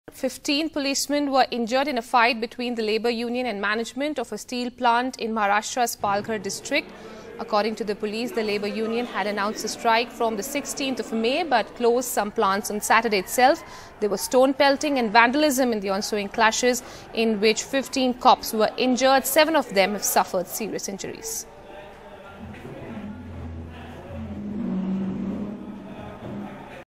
Several Cops Injured During Clashes In Maharashtra Steel Factory; 27 Arrested
Several policemen were injured and 12 of their vehicles damaged when they tried to control attack on employees of a steel company by over 100 members of a labour union here in Maharashtra, an official said on Sunday.